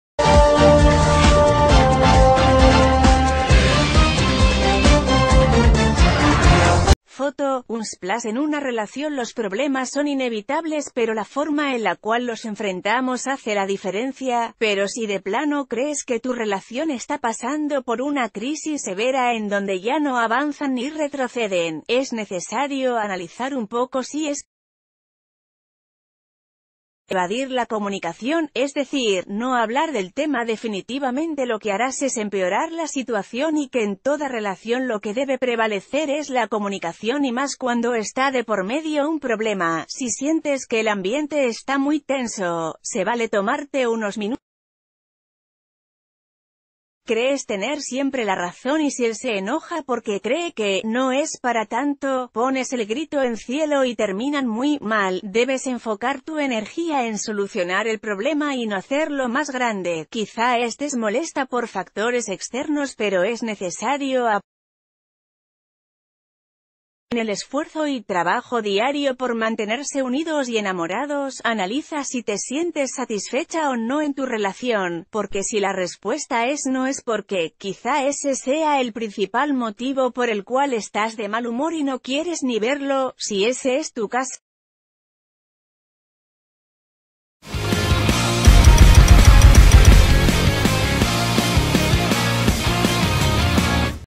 ¡El problema en tu relación no es él, eres tú! Tres señales que lo demuestran
Foto: Unsplash En una relación los problemas son inevitables pero la forma en la cual los enfrentamos hace la diferencia, pero si de plano crees que tu relación está pasando por una crisis severa en donde ya no avanzan ni retroceden, es necesario analizar un poco si ese “estancamiento” es solo por é...